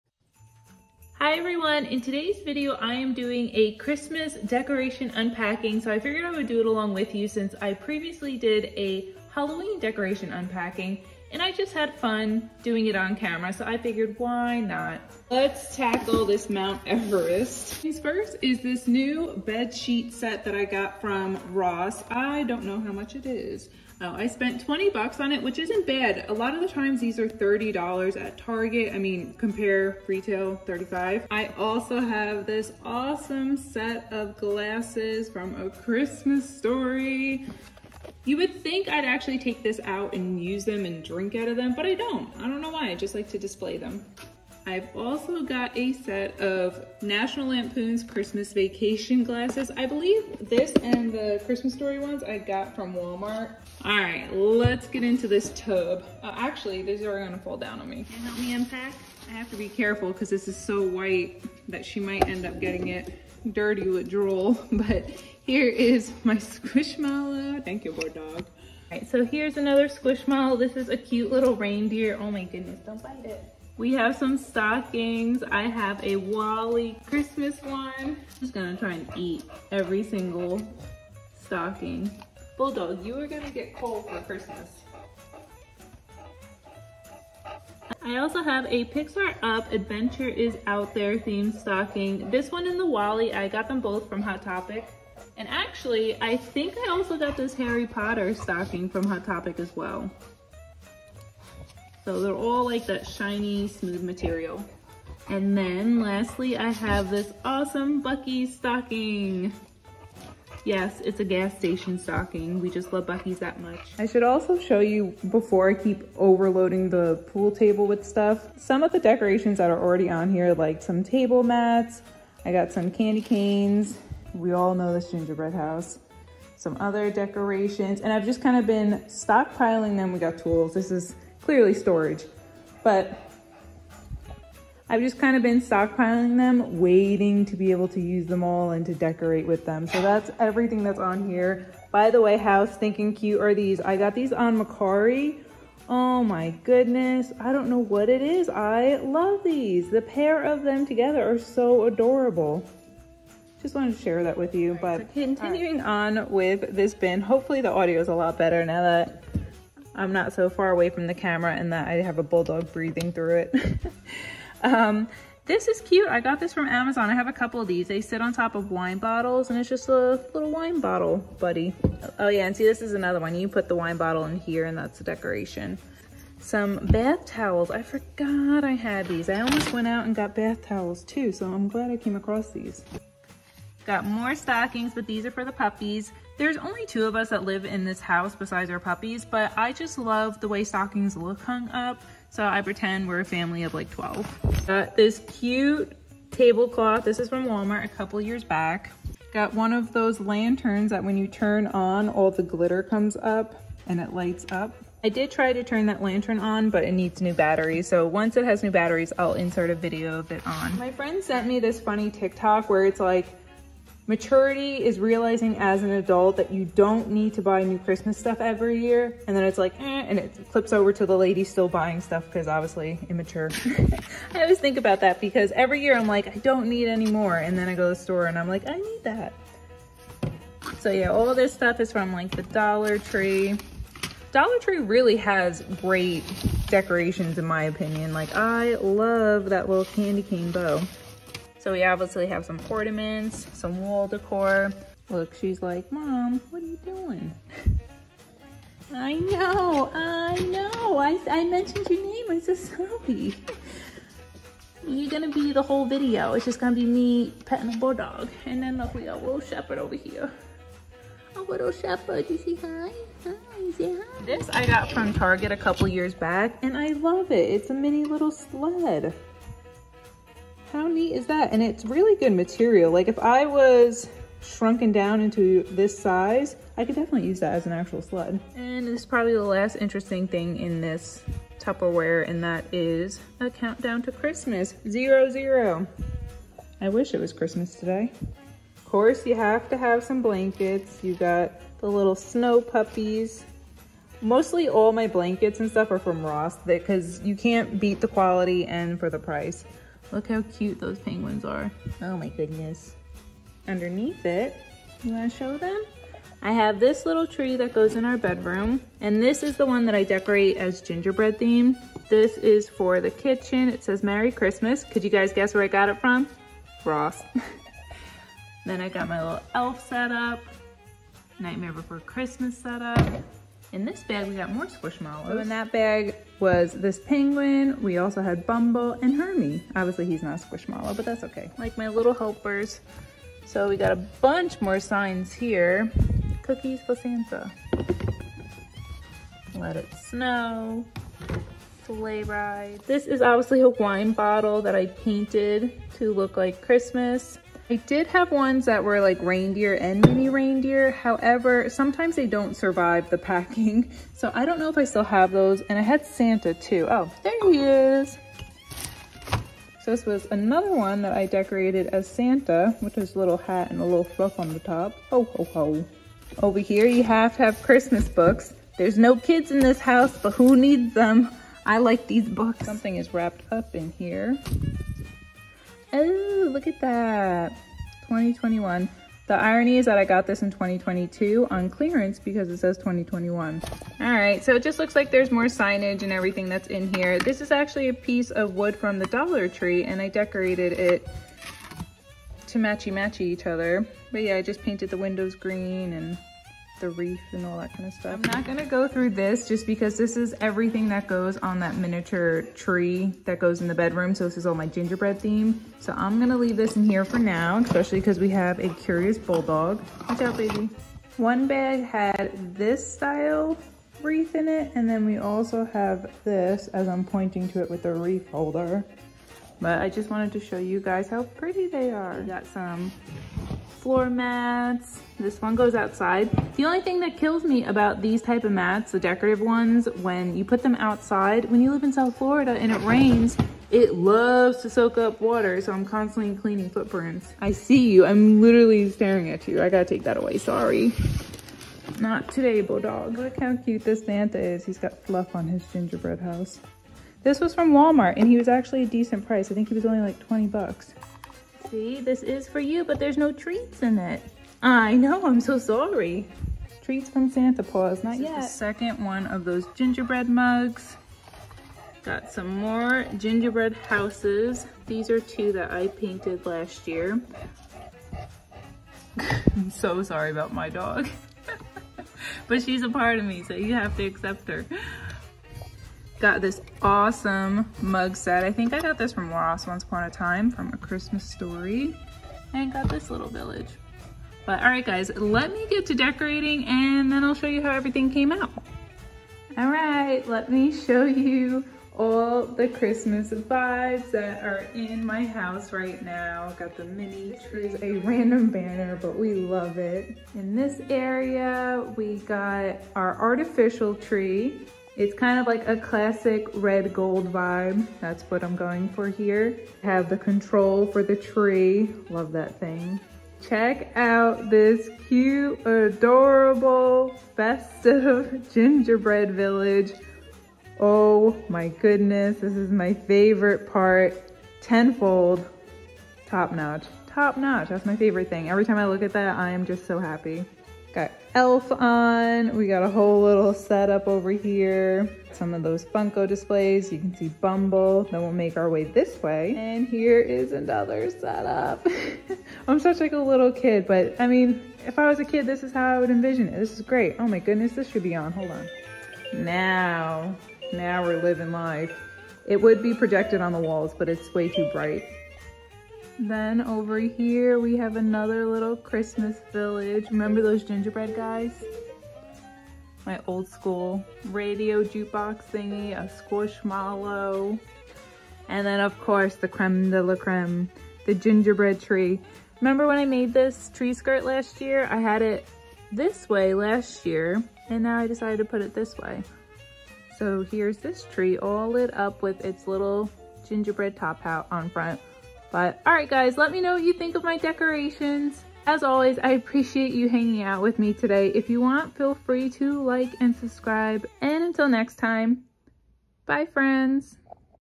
Decorating for Christmas!
It’s that time of year again! Let’s hangout and unpack my Christmas decorations and get ready for Christmas! ￼

Fabri-Tac Permanent Adhesive:

Spatula Daler-Rowney Simply Brown Artist Palette Knife

Dental pick for tucking

Tiny Curved scissors for trimming:

Odicoat fabric waterproof treatment

White leather paint:

Black leather paint:

Acrylic Finisher: